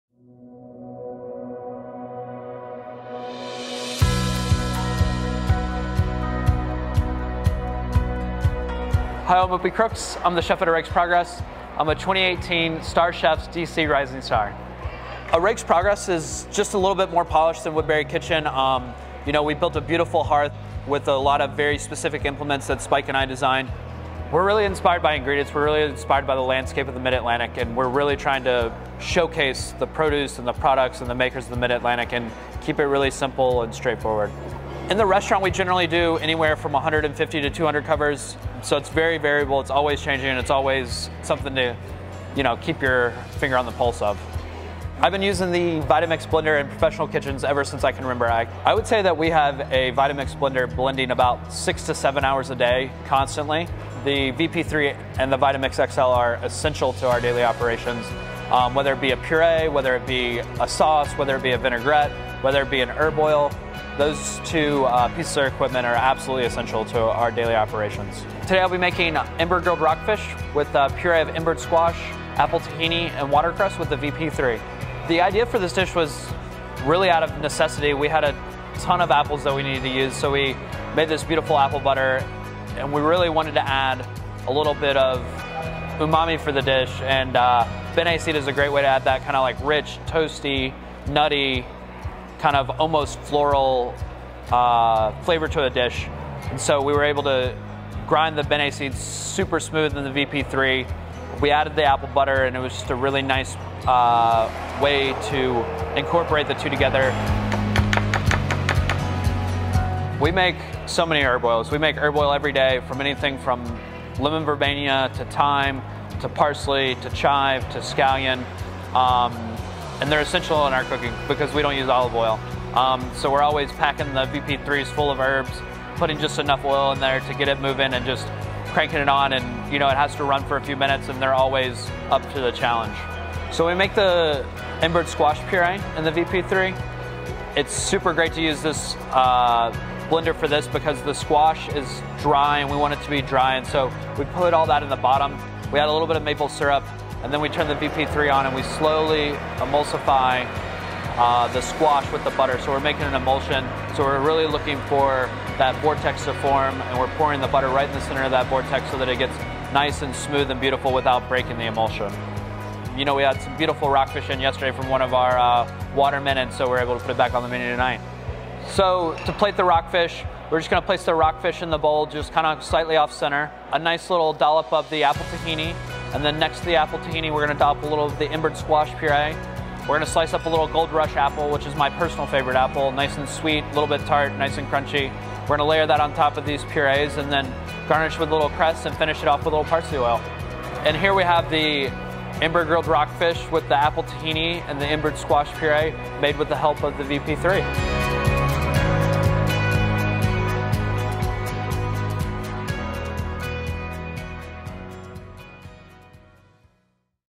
Chef Opie Crooks | A Rake's Progress
2018 Washington D.C. Rising Star Chef Opie Crooks of A Rake's Progress shows us how he uses the Vitamix Vita-Prep 3 in his kitchen to make this delicious grilled rockfish with embered squash puree and  apple tahini.